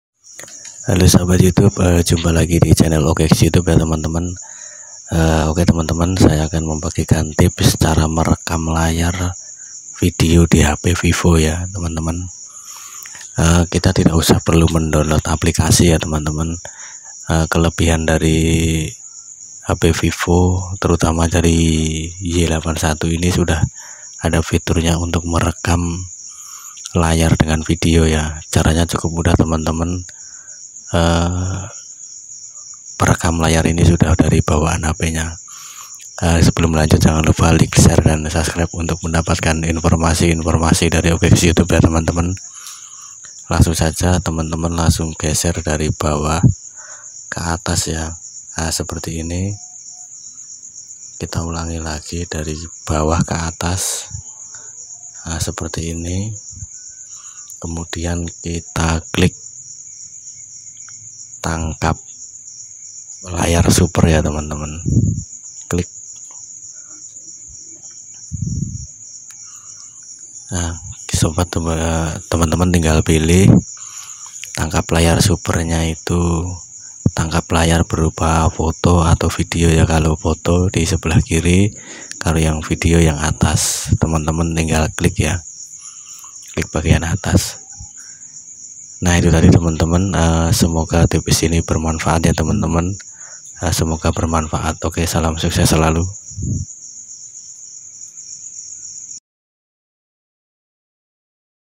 Cara merekam vidio di layar hp android vivo
Cara merekam vidio layar hp android vivo

Tips merekam layar vidio di hp android vivo tanpa aplikasi khusus. kelebihan hp android vivo yang jarang di miliki android lain yakni merekam layar video pada layar.